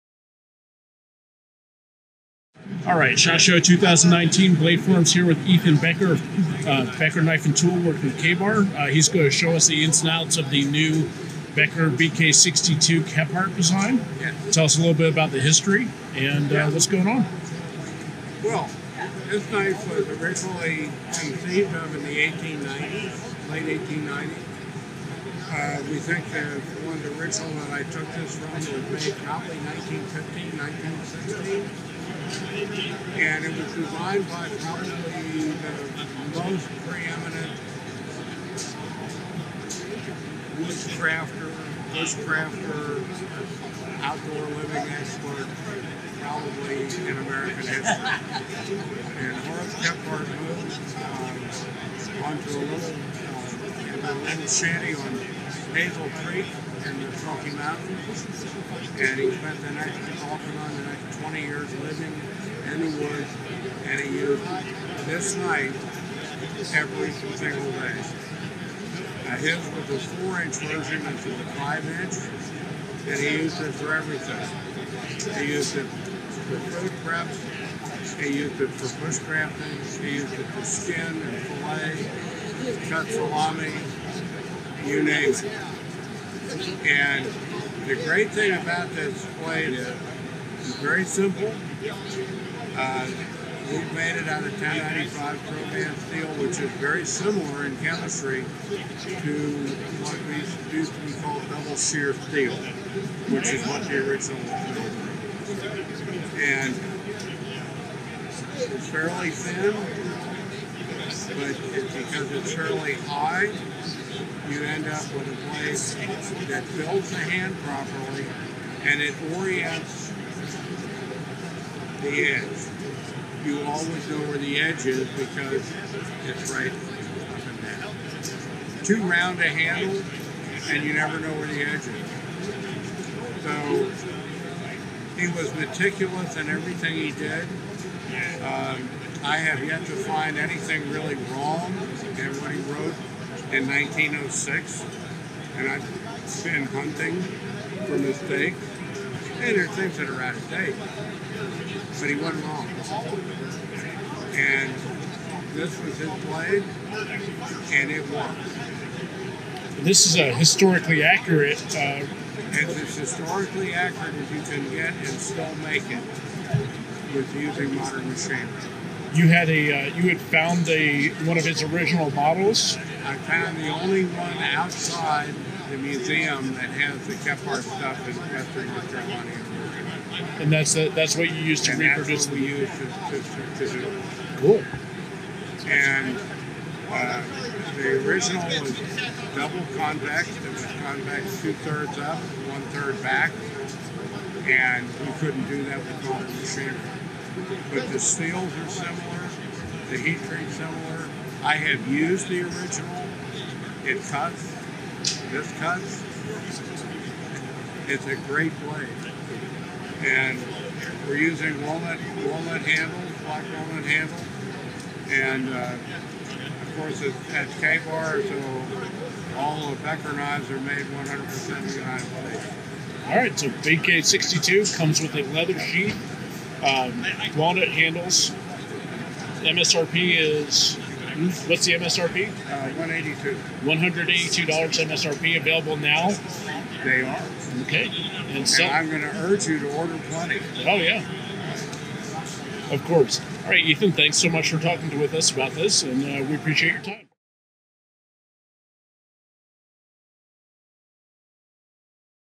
SHOT Show 2019 - Ethan Becker discusses the BK62 Kephart
Ethan Becker discusses the design philosophy and history behind the newest Becker Knife & Tool offering, the BK 62 Kephart